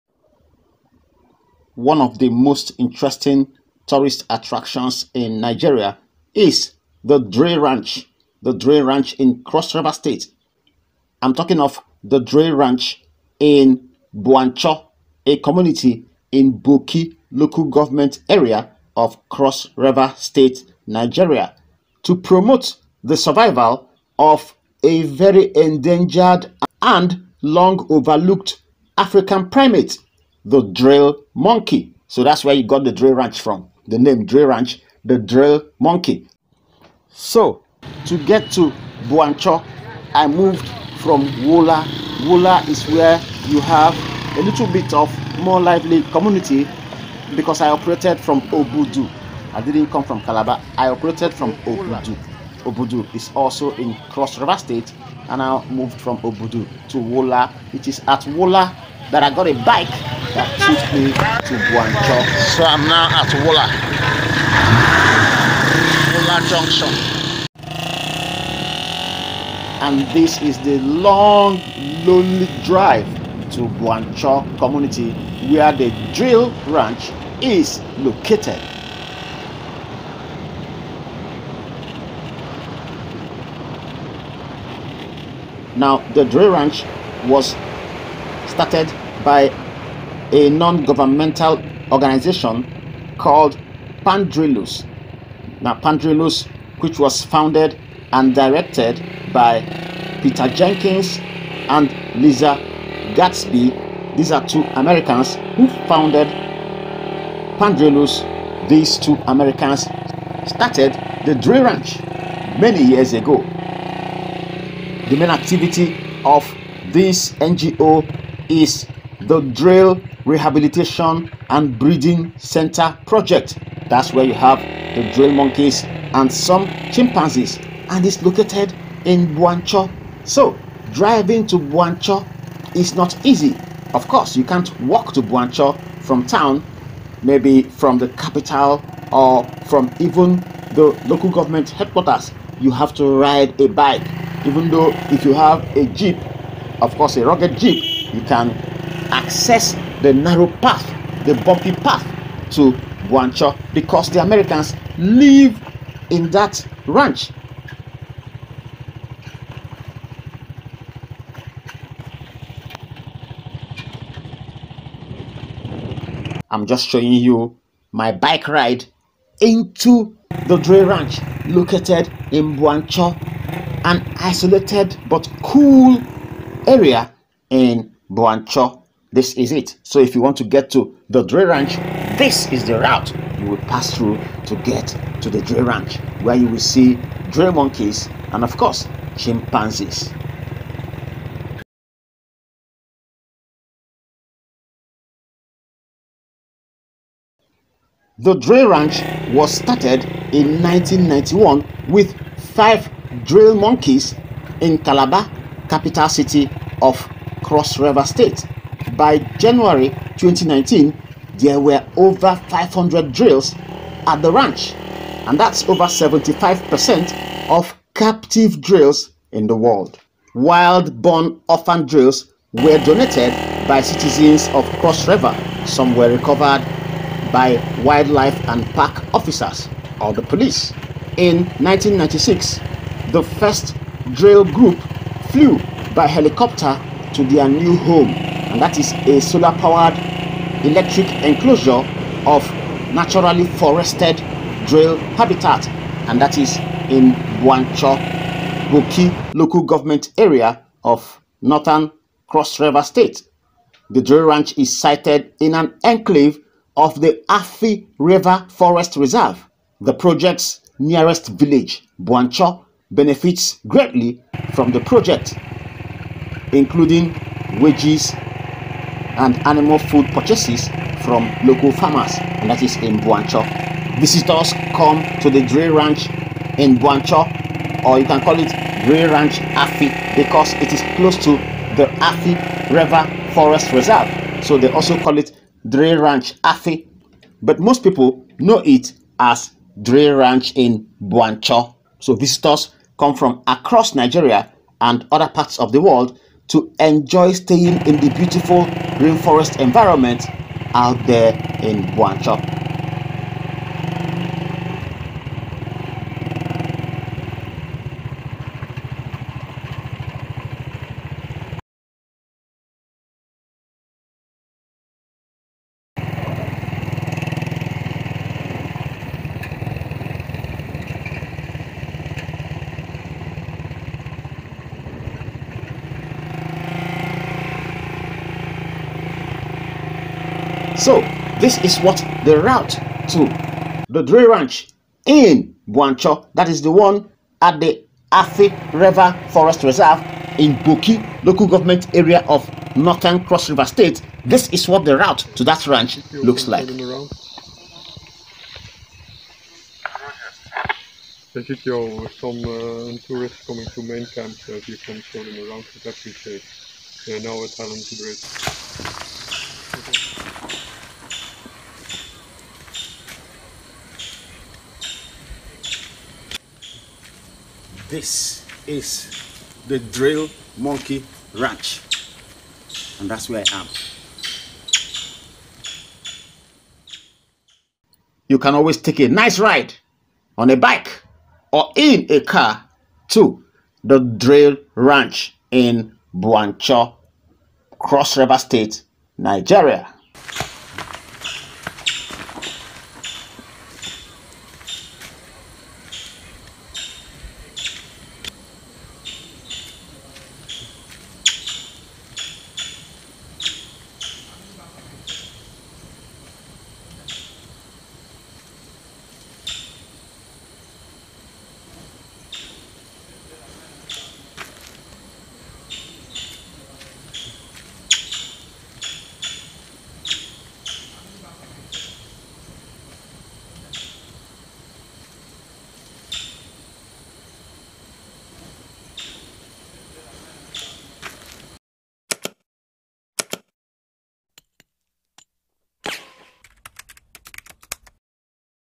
American guard stops me in monkey forest
I rode on a bike into the thick forest where some Americans are operating a project that conserves endangered specie of monkeys